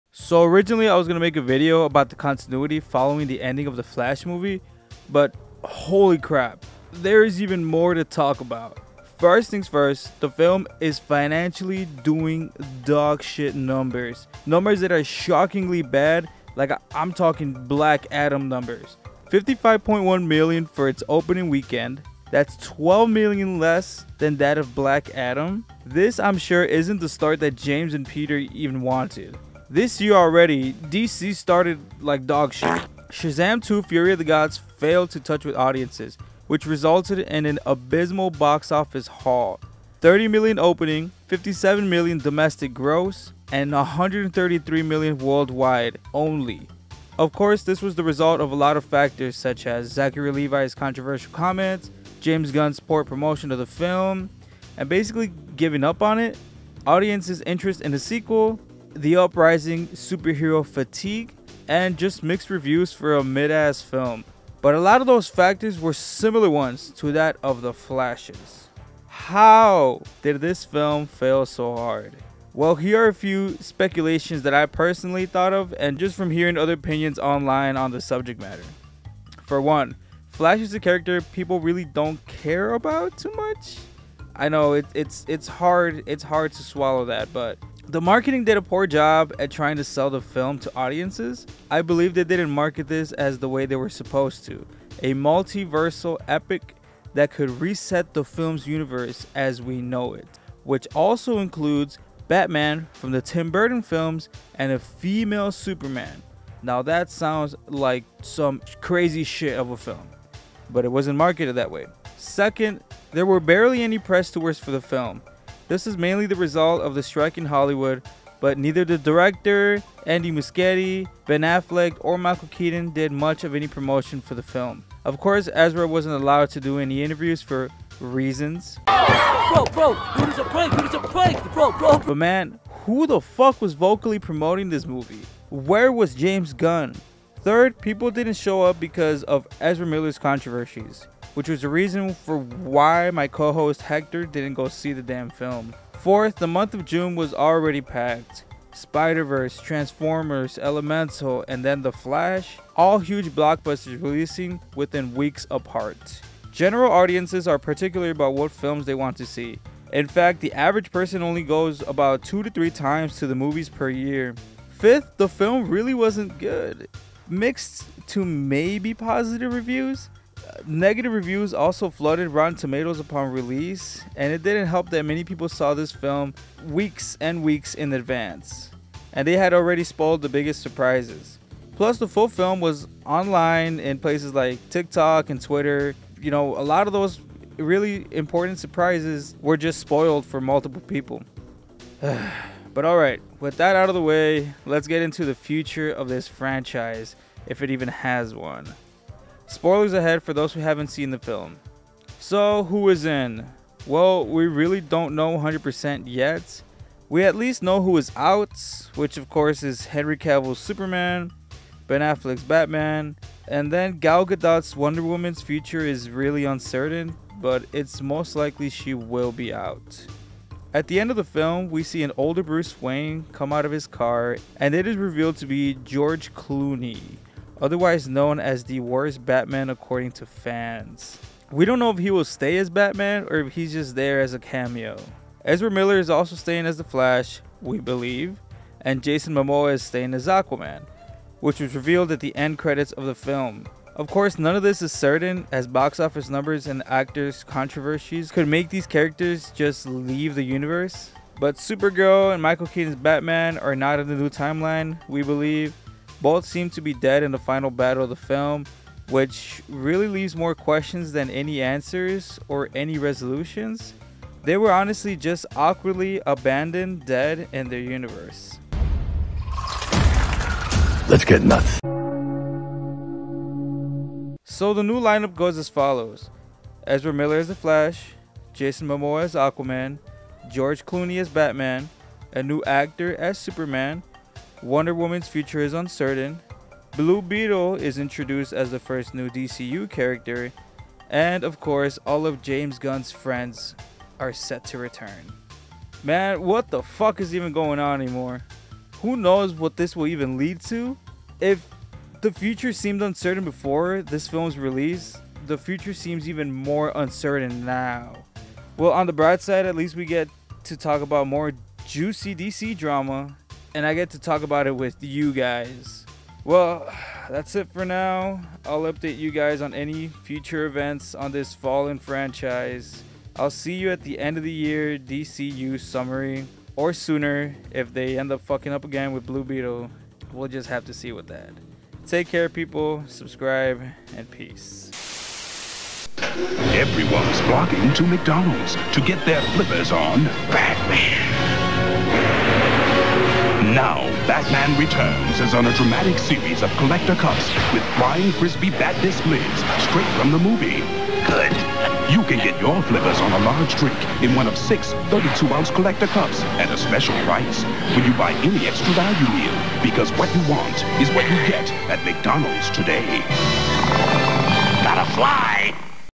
WTF is going on with The Flash!
Overdue video but I wanted to talk about some of the latest DC drama revolving the flash film. Trust me its looking worse than ever. I recorded this a week ago before the second box office numbers officially came out. Also yes I know the casting for Superman and Lois Lane was revealed. Enjoy the video and let me know of your thoughts as to why the film failed.

Also I just realized that youtuber Zeepsterd literally made a Flash video talking about some of the same points in this one. Just know that I DID NOT COPY OK DAMN LOL. This video literally was being uploaded as the other released. He just beat me to the punch. But then again this is just a trending topic of discussion.

One more thing. Yes I know that this type of thumbnail has been overused and done to death, but it brings in the clicks so yeah.